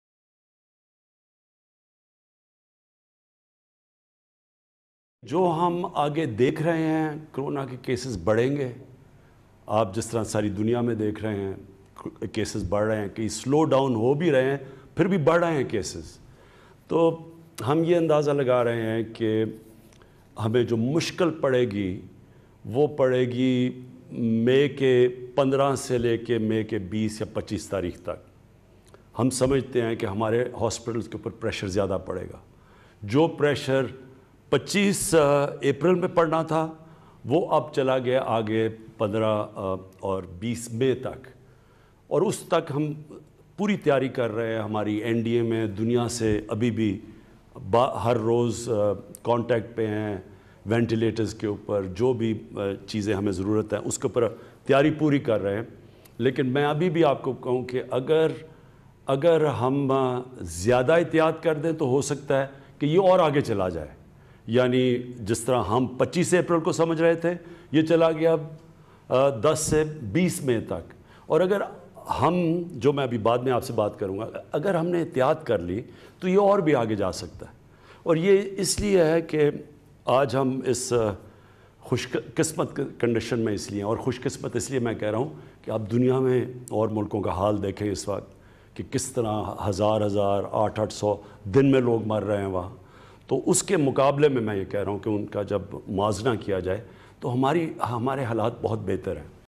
دنیا میں وباء کا پھیلاؤ جس تیزی سے بڑھ رہا ہے پاکستان میں بھی بڑی تعداد متوقع تھی لیکن ہم اپنی احتیا
دنیا میں وباء کا پھیلاؤ جس تیزی سے بڑھ رہا ہے پاکستان میں بھی بڑی تعداد متوقع تھی لیکن ہم اپنی احتیاطی تدابیر کی باعث اسے قابو کرنے میں اب تک کامیاب رہے ہیں۔NDMA پوری دنیا سے رابطے میں ہے اور مزید طبی سامان خرید رہے ہیں۔مزید احتیاط ہمیں مشکلات سے بچا سکتی ہے
وزیراعظم پاکستان